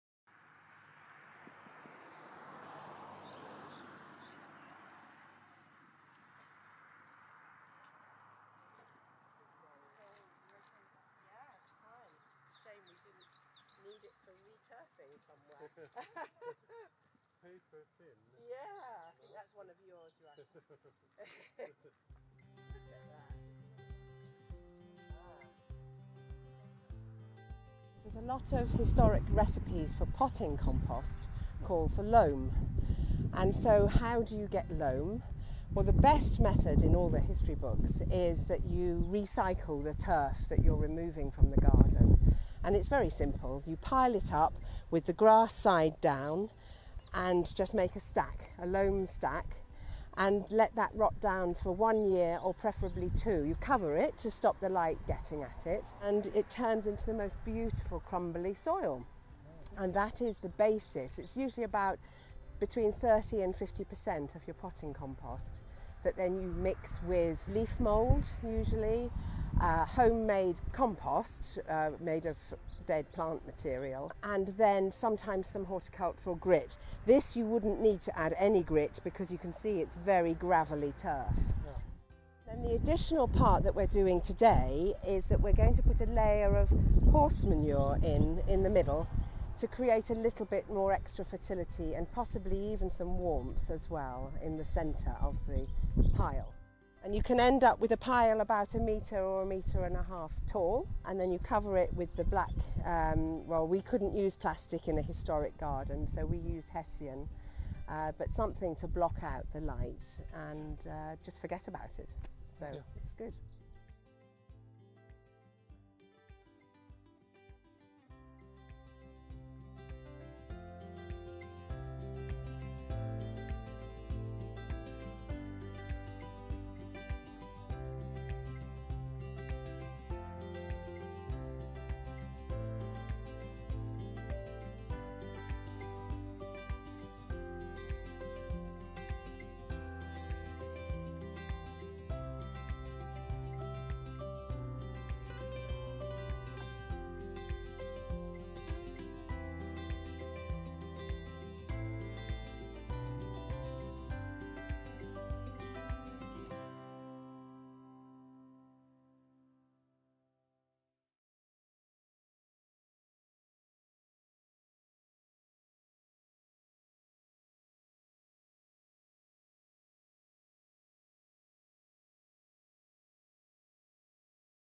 Soil Management in Historic Gardens-workshop: The turf session
The turf session - from the Soil Management-workshop, a part of the Erasmus+ funded project Craft Skills for Garden Conservation.